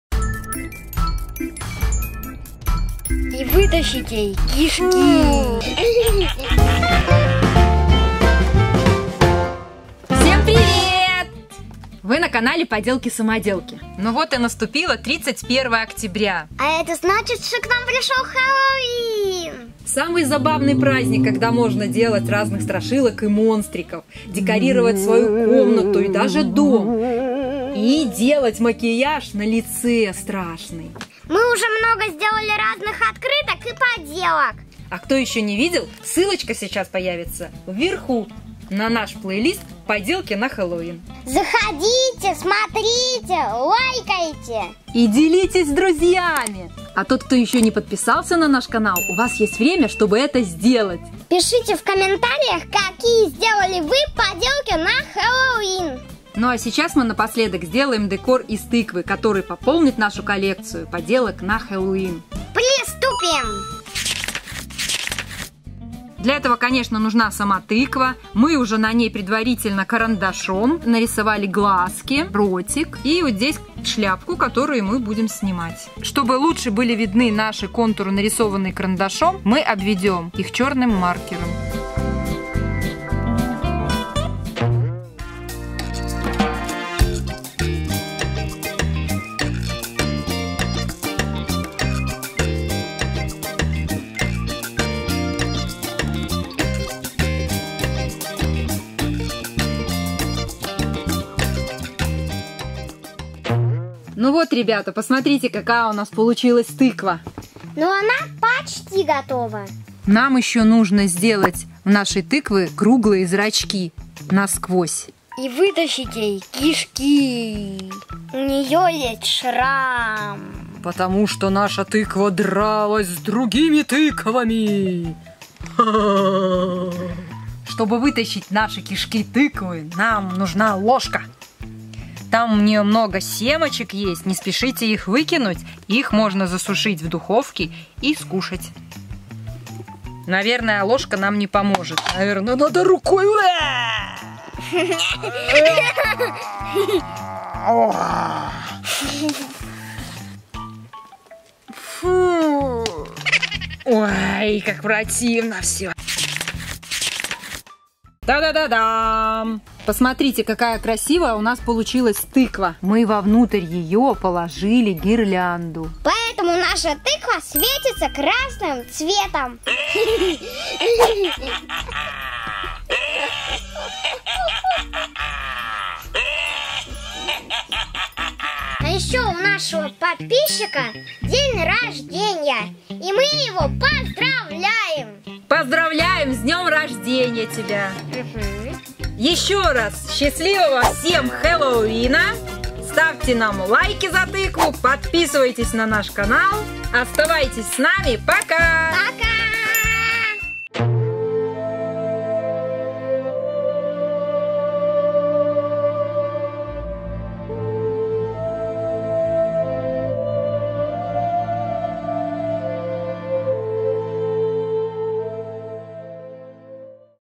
Декор Тыква на Хэллоуин своими руками. Как сделать Фонарик из Тыквы. Поделку на Helleween. DIY
В нашем видео вы сможете научится делать декор на Хэллоуин - это самая важная деталь этого праздника. Светящаяся тыква фонариком с устрашающей улыбкой.

Счастливого Хеллоуина, друзья!